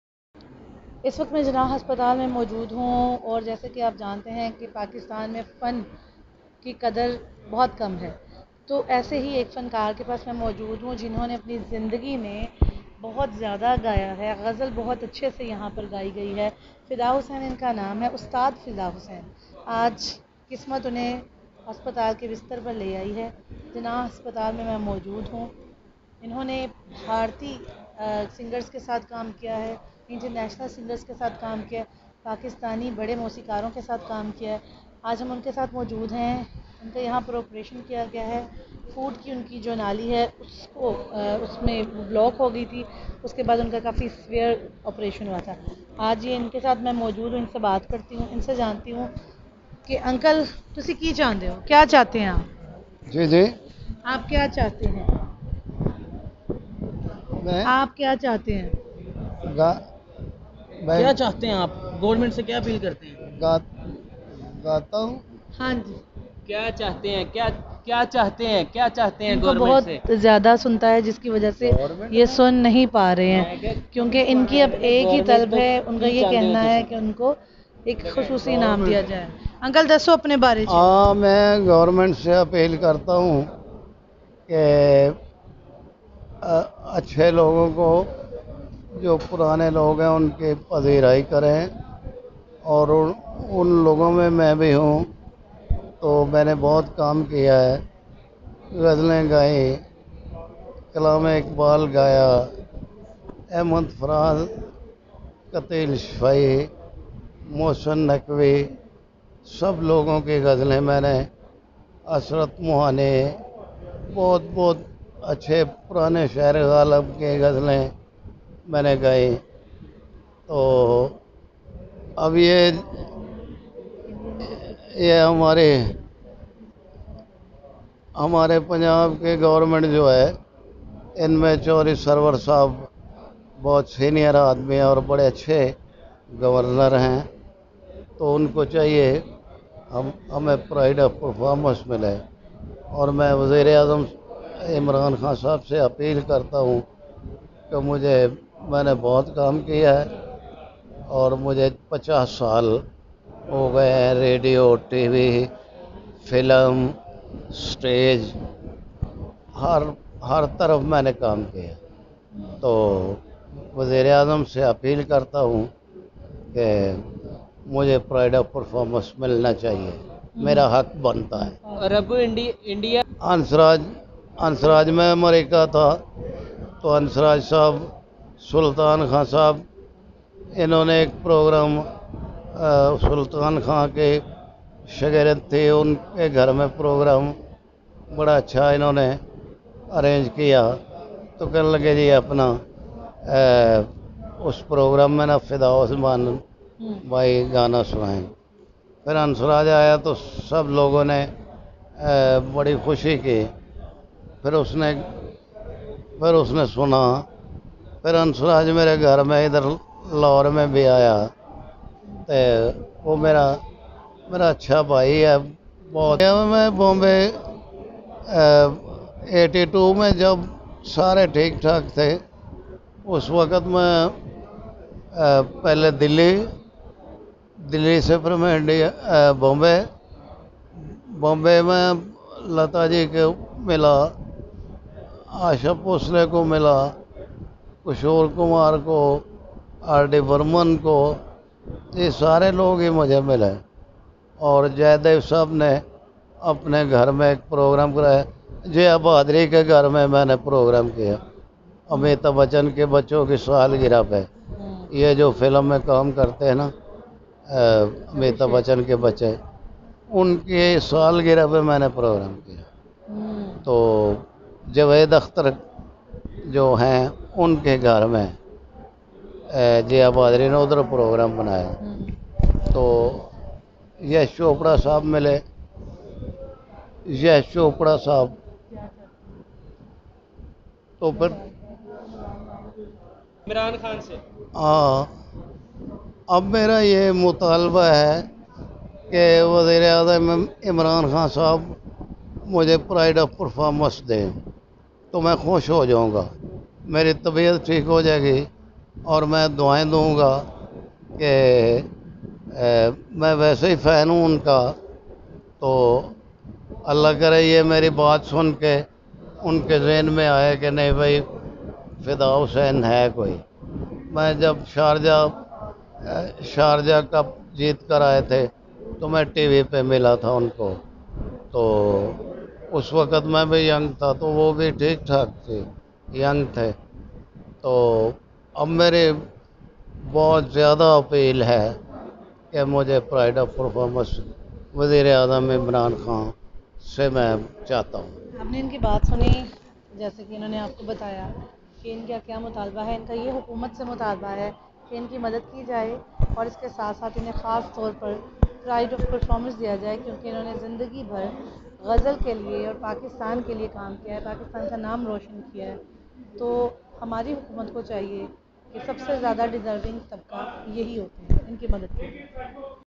Ustad Fida Hussain passed away seeking Goverenmnt help | His Last Interview | Afifa Nasar Ullah
The Poor singer has Passed away seeking Pride of Performance award

Ustad Fida Hussain was a Legend Gazal Go Artist who had been one of the famous Gazal Goes whose shows were telecasted on radio and Television. He had worked for more than fifty years to the country. He had worked with the most famous singers of India like Lata G, Aasha Bhosley, RD Barman and many more. He had also worked with Pakistani Big names including Ustad Gulam Ali and many more.

The legend Artist died seeking help from Government.

He had been very sick and was admitted to Jinnah Hospital because of Blockage in Food Pipe. He had been operated.

No one from Government came to console him.He died Seeking Pride of Performance.

Here he remained waiting for the positive stance taken by Government. He wished  that government, specially PM Imran Khan and Governer CH. Muhammad Sarwar award him with Pride of Proformance award because of his utmost and hard work through out his whole life.